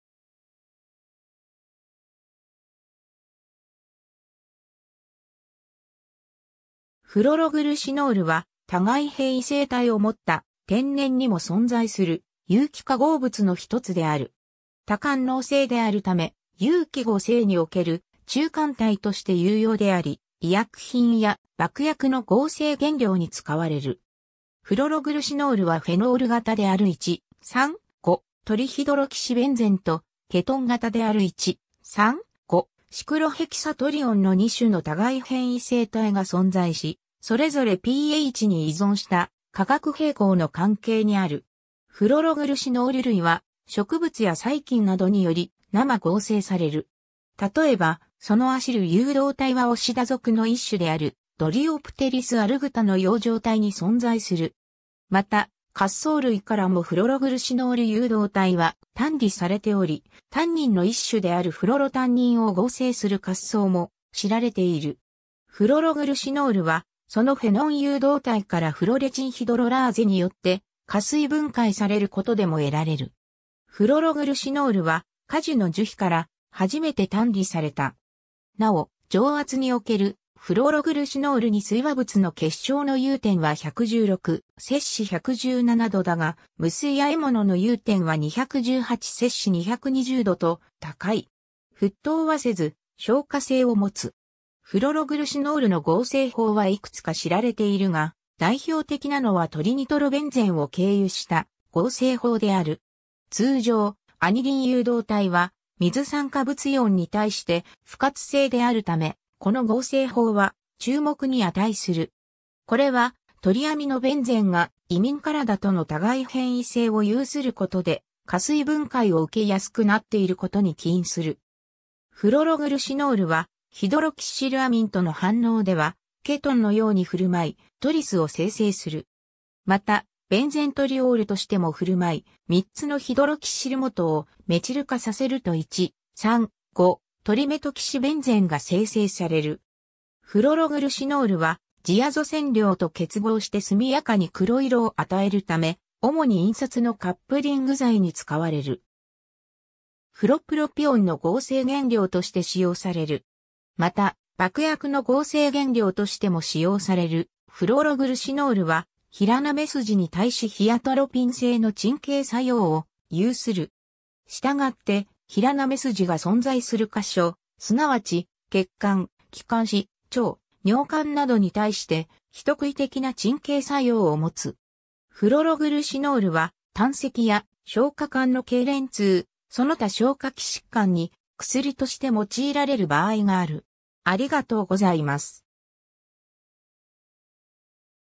フロログルシノール
フロログルシノール（phloroglucinol）は、互変異性体を持った、天然にも存在する有機化合物の1つである。
多官能性であるため、有機合成における中間体として有用であり、医薬品や爆薬の合成原料に使われる。
 フロログルシノールはフェノール型である1,3,5-トリヒドロキシベンゼンと、ケトン型である1,3,5-シクロヘキサトリオン（フロログルシン）の2種の互変異性体が存在し、それぞれpHに依存した化学平衡の関係にある。
 フロログルシノール類は、植物や細菌などにより生合成される。
例えば、そのアシル誘導体はオシダ属の1種であるドリオプテリス・アルグタの葉状体に存在する。
また、褐藻類からもフロログルシノール誘導体は単離されており、タンニンの1種であるフロロタンニンを合成する褐藻も知られている。
 フロログルシノールは、そのフェノン誘導体からフロレチンヒドロラーゼによって加水分解されることでも得られる。
 フロログルシノールは、果樹の樹皮から初めて単離された。
なお、常圧におけるフロログルシノール2水和物の結晶の融点は116-117 °Cだが、無水和物の融点は218-220 °Cと高い。
沸騰はせず、昇華性を持つ。
 フロログルシノールの合成法は幾つか知られているが、代表的なのはトリニトロベンゼンを経由した合成法である。
 通常、アニリン誘導体は水酸化物イオンに対して不活性であるため、この合成法は注目に値する。
これは、トリアミノベンゼンがイミン体との互変異性を有することで加水分解を受け易くなっていることに起因する。
 フロログルシノールは、ヒドロキシルアミンとの反応ではケトンのように振る舞い、トリス(オキシム)を生成する。
また、ベンゼントリオール(Ka1 = 3.56 × 10−9, Ka2 1.32×10−9)としても振る舞い、3つのヒドロキシル基をメチル化させると1,3,5-トリメトキシベンゼンが生成される。
 フロログルシノールはジアゾ染料と結合して速やかに黒色を与えるため、主に印刷のカップリング剤に使われる。
 フロプロピオンの合成原料として使用される。
 また、爆薬の合成原料としても使用される フロログルシノールは、平滑筋に対し非アトロピン性の鎮痙作用を有する。
したがって、平滑筋が存在する箇所、すなわち、血管、気管支、腸、尿管などに対して、非特異的な鎮痙作用を持つ。
 フロログルシノールは胆石や消化管の痙攣痛、その他消化器疾患に、薬として用いられる場合がある。